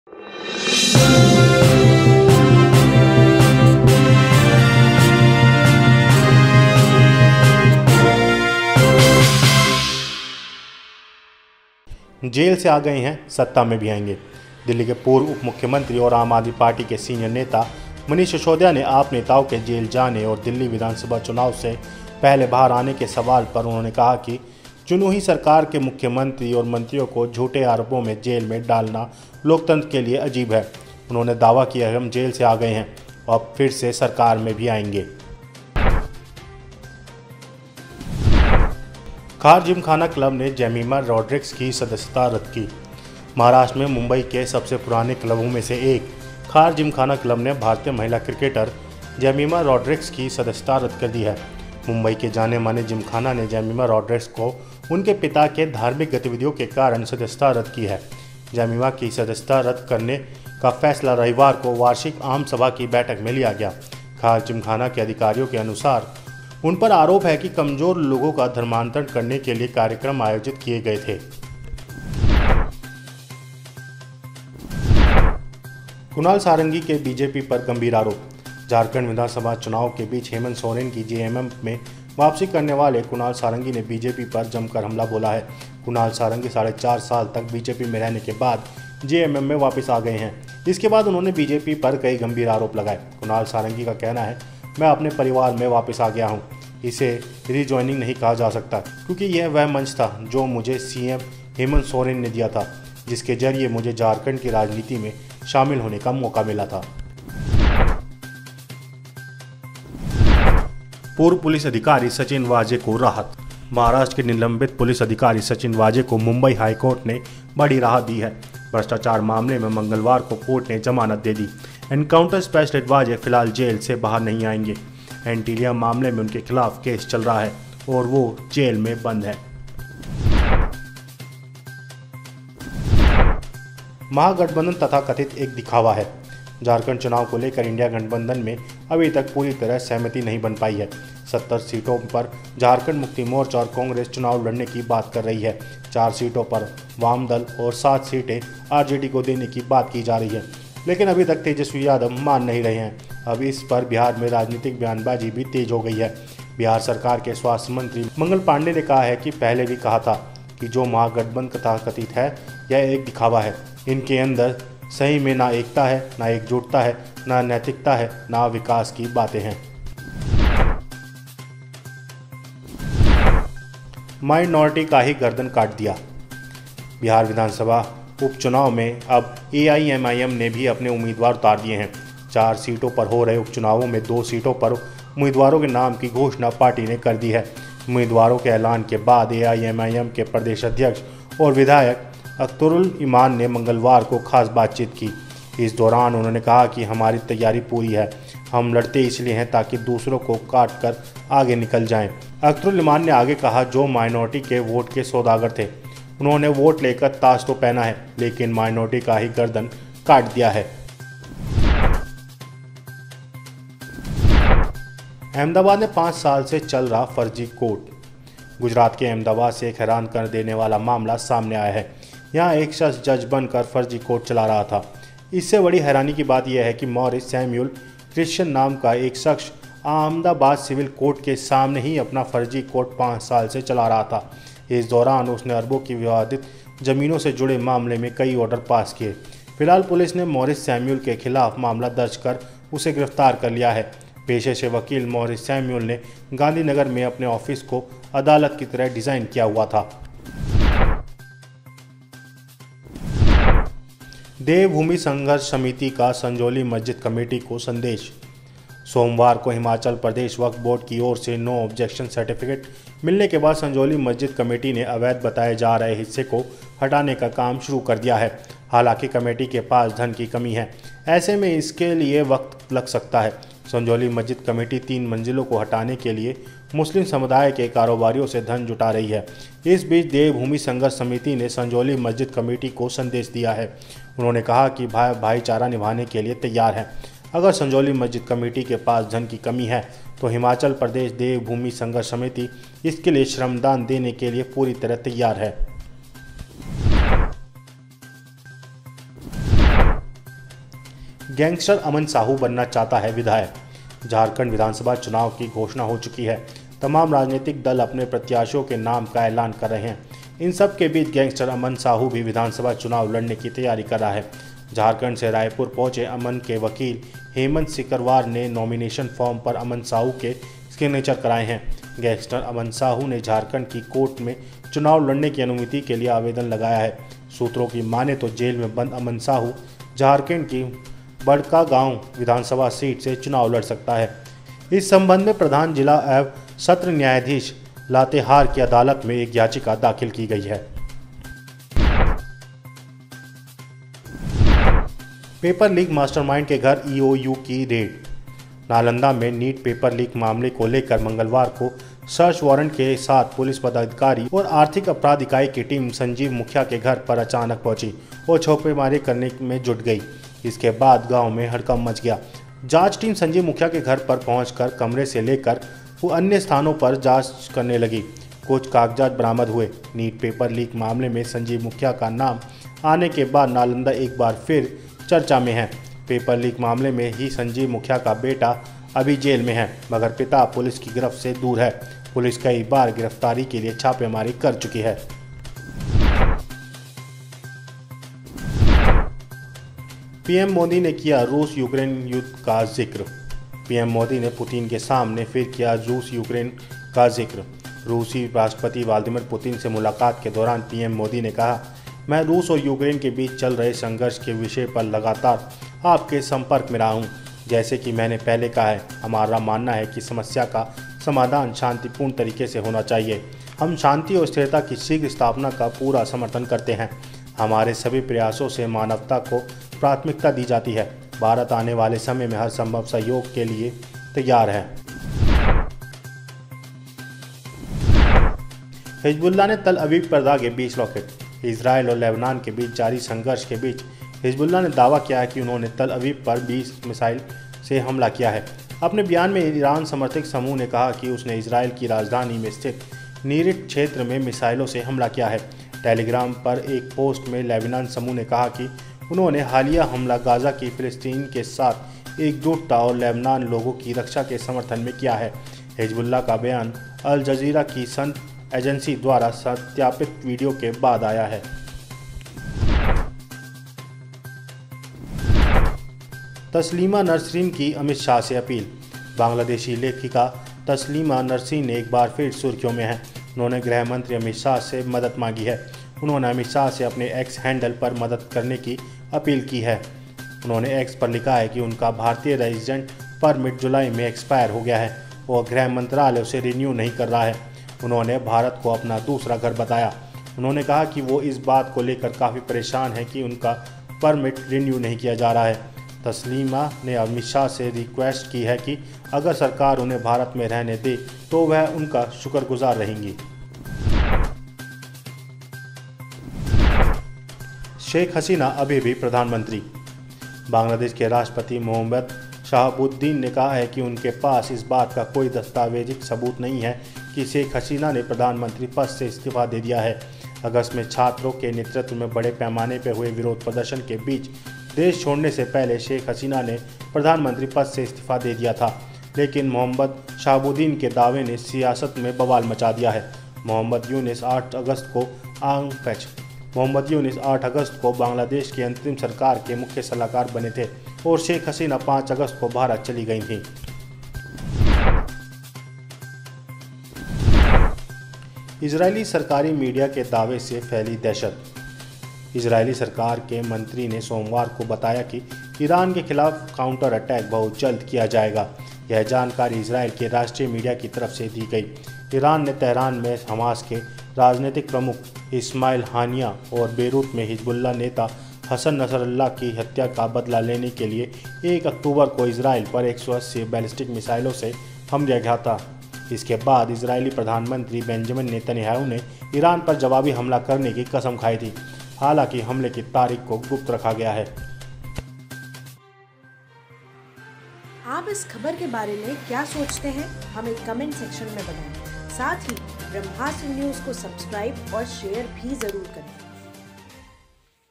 शेख हसीना अब भी प्रधानमंत्री? | 'National-International' Bulletin
1. जेल से आए गए हैं, सत्ता में भी आएंगे
2.  खार जिमखाना क्लब ने जेमिमा रोड्रिग्स की सदस्यता रद्द की
3. कुणाल सारंगी के BJP पर गंभीर आरोप
4. पूर्व पुलिस अधिकारी सचिन वाजे को राहत
5. महागठबंधन तथाकथित एक दिखावा है
6. माइनॉरिटी का ही गर्दन काट दिया
7.  अहमदाबाद में पांच साल से चल रहा फ़र्ज़ी कोर्ट
8.  देवभूमि संघर्ष समिति का संजौली मस्जिद कमेटी को संदेश
9. गैंगस्टर अमन साहू बनना चाहता है विधायक
10. पेपर लीक मास्टरमाइंड के घर EOU की रेड
11.  पीएम मोदी ने किया रूस-यूक्रेन का जिक्र,को तैयार
12. हिजबुल्लाह ने तेल अवीव पर दागे 20 रॉकेट
13. तसलीमा नसरीन की अमित शाह से अपील
14. शेख हसीना अब भी प्रधानमंत्री?
15. इजरायली सरकारी मीडिया के दावे से फैली दहशत